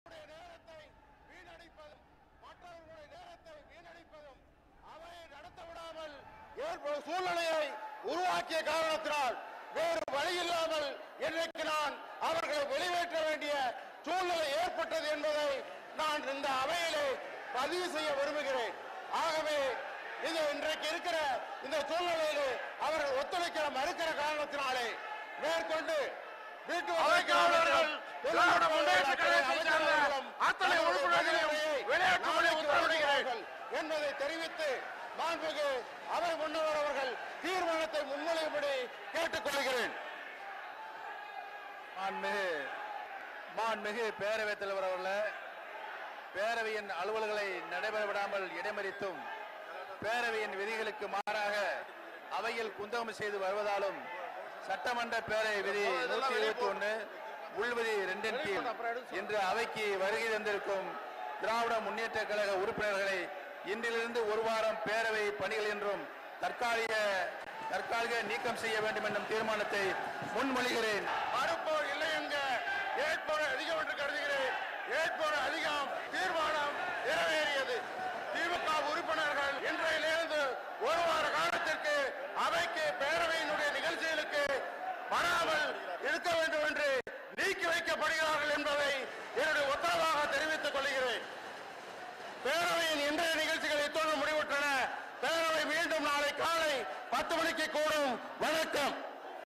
DMK MLAs Suspended, Forcibly Evicted from Tamil Nadu Assemebly | News18 TamilNadu
Tamil Nadu Assembly Speaker on Wednesday suspended  opposition DMK MLAs from the house for a week after they strongly protested against remarks made by a ruling AIADMK member.